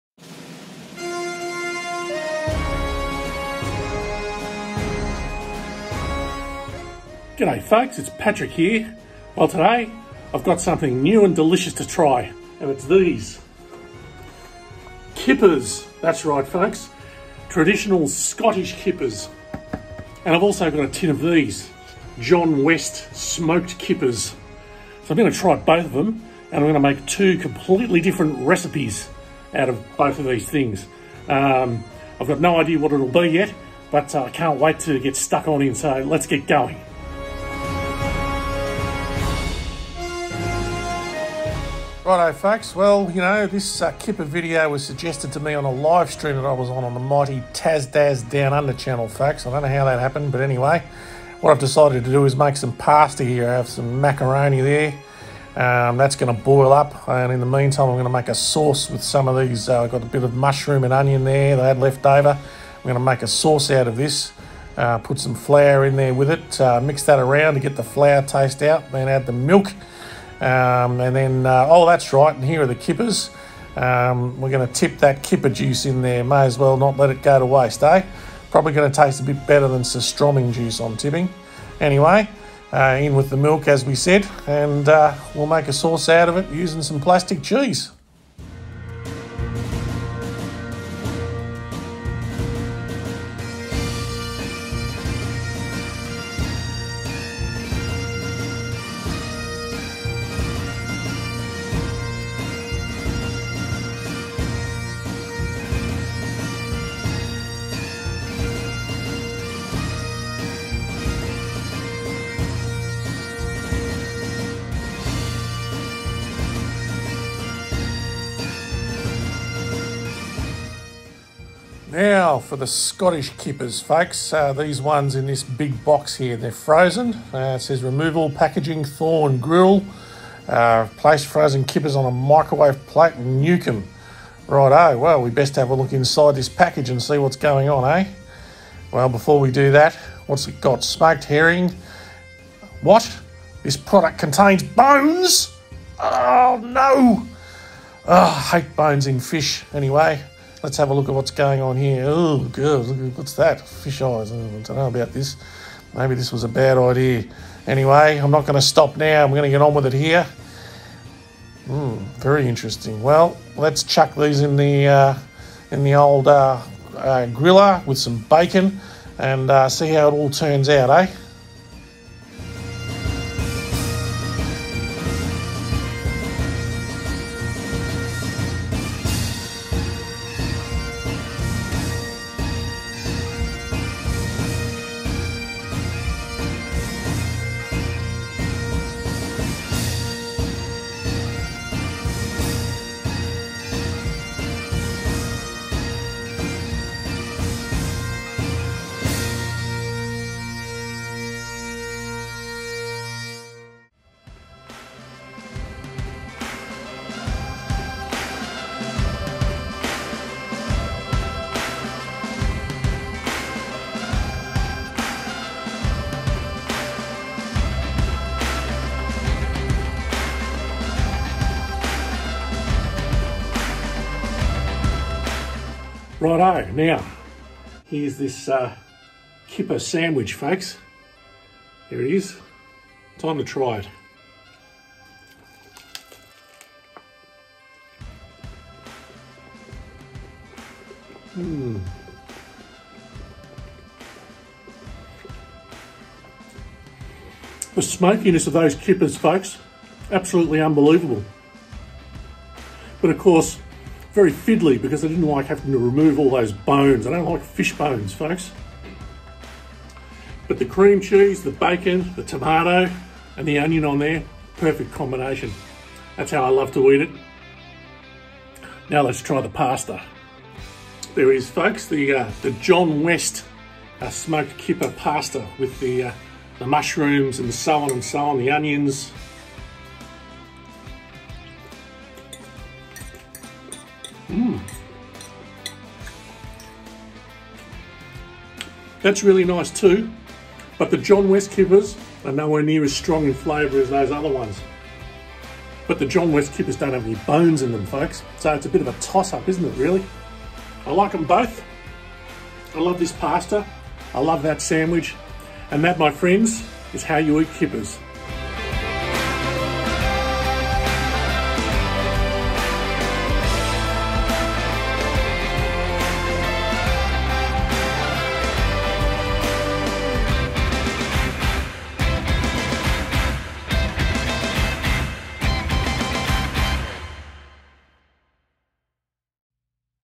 How To Eat Kippers 🐟 Scottish Kippers
I love kippers and I could eat them all day!

Cheers to Tasdaz Downunder for suggesting this check out Tasdaz Downunder here...